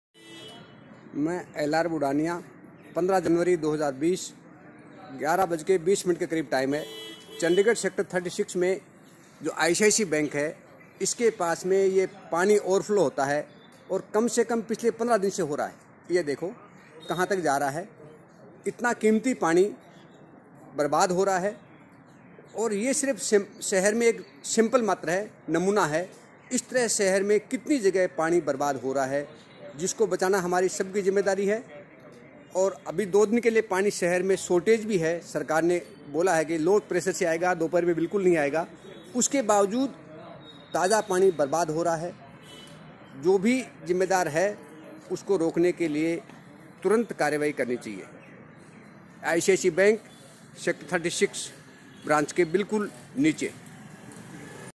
Save water save environment stop leakage(1)
This is only 1 or 2 sample all city same condition everyday wastage of water by leakage need immediate action stop leakage save water save environment